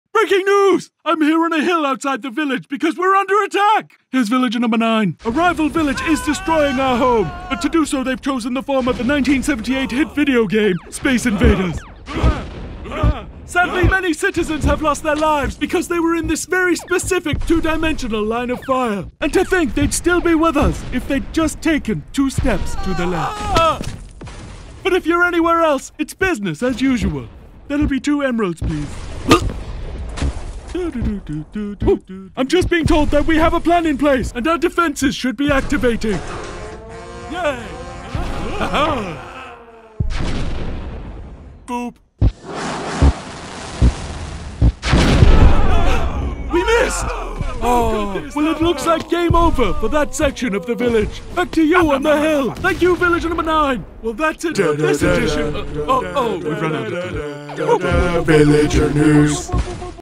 VILLAGER NEWS: INVASION! (Minecraft Animation)
The Villager News crew jump into action when the village is attacked by Invaders! Can the village defend itself against this ancient threat!?